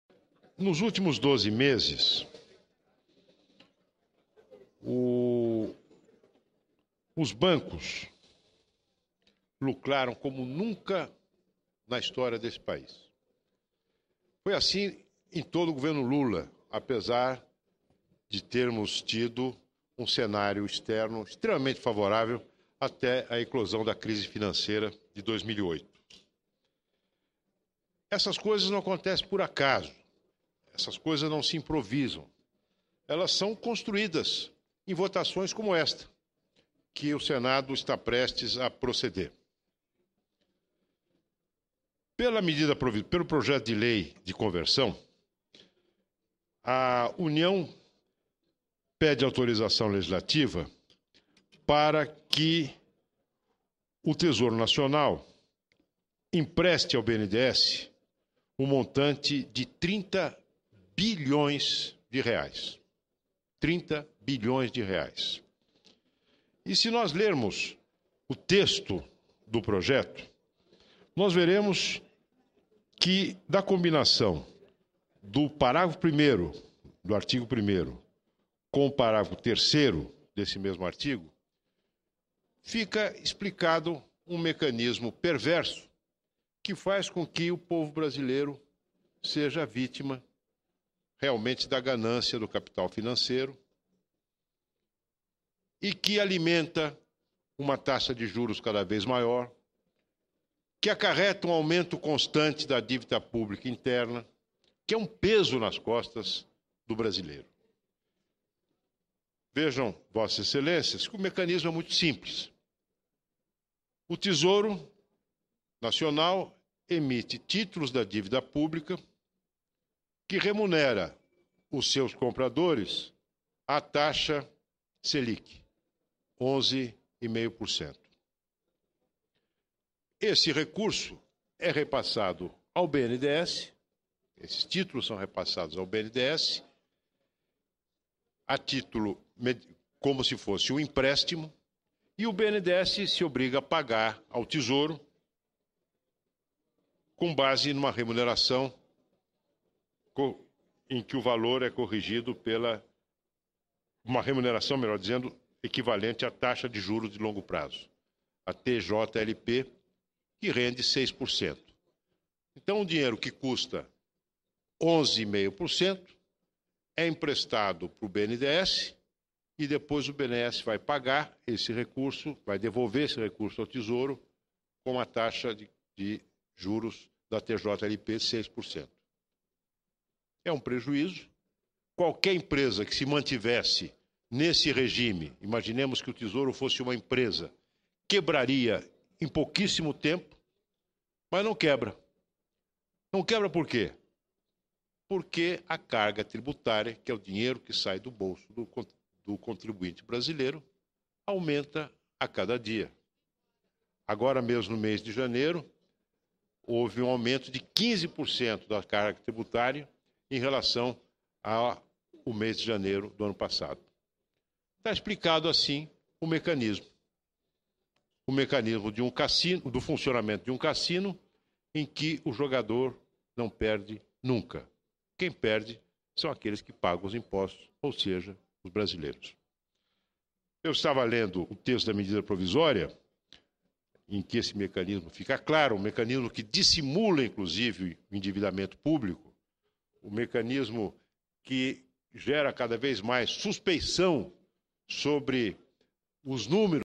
Sen. Aloysio Nunes Ferreira contesta aumento da dívida pública para financiar o BNDES
01/03/2011 - Plenário - Sen. Aloysio Nunes Ferreira contesta aumento da dívida pública para financiar o BNDES